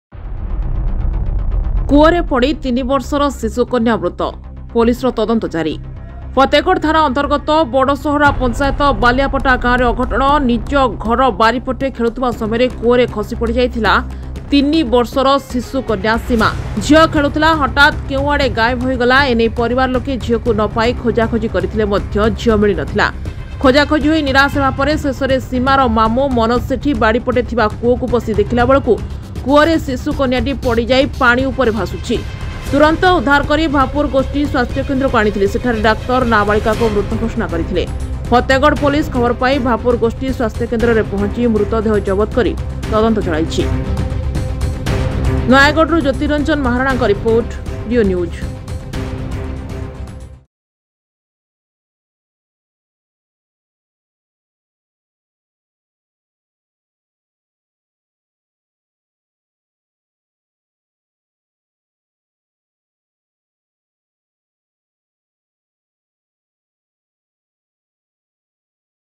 କୂଅରେ ପଡ଼ି ତିନିବର୍ଷର ଶିଶୁକନ୍ୟା ମୃତ୍ୟୁ
କୂଅରେ ପଡ଼ି ତିନିବର୍ଷର ଶିଶୁକନ୍ୟା ମୃତ୍ୟୁReonews